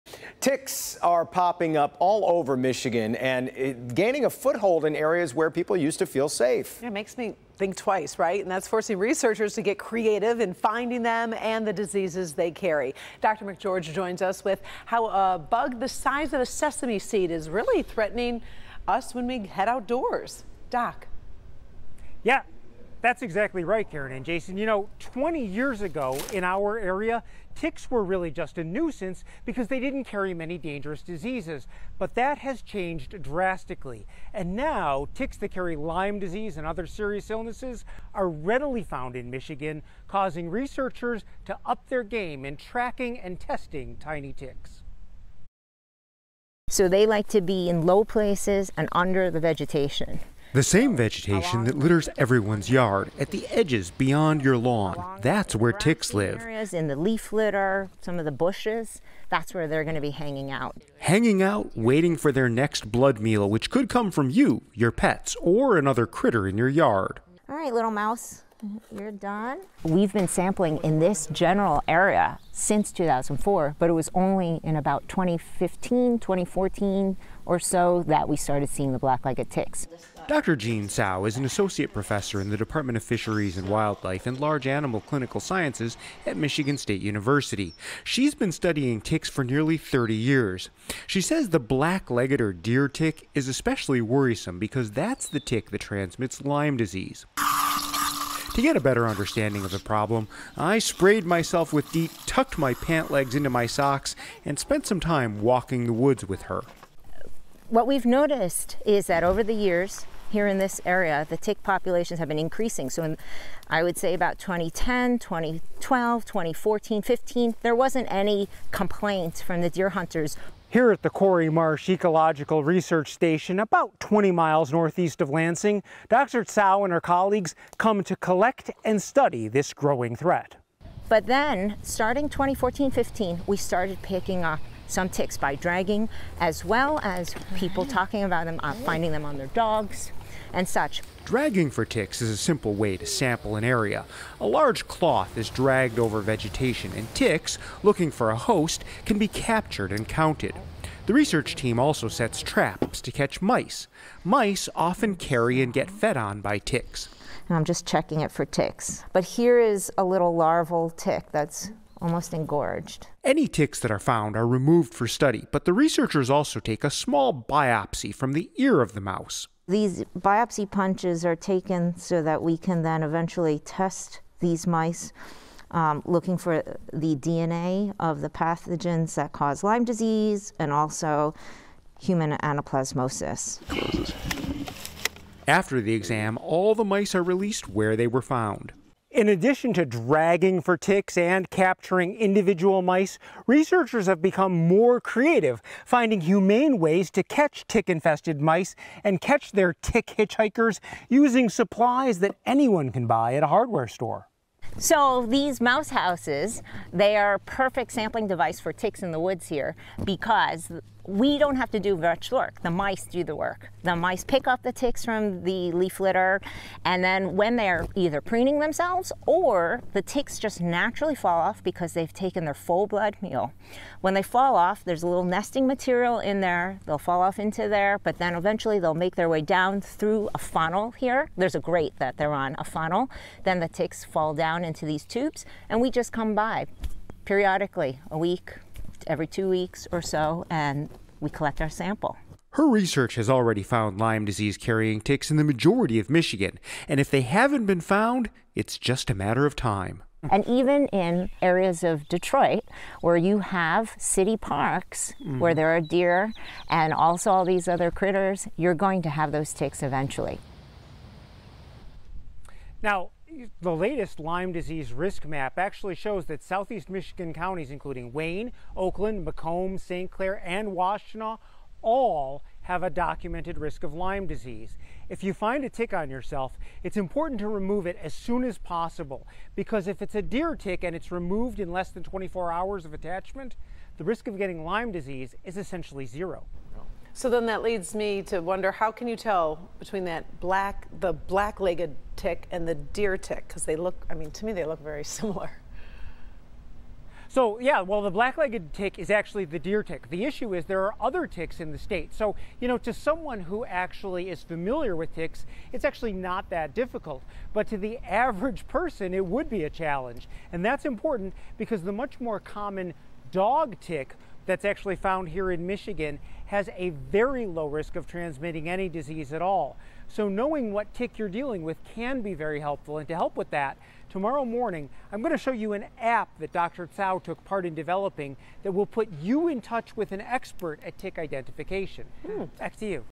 Tick expert from Michigan State University weighs in on what you need to know when you encounter...
20 years ago, ticks in our area were more of a nuisance because they rarely carried serious diseases. Unfortunately, that has changed.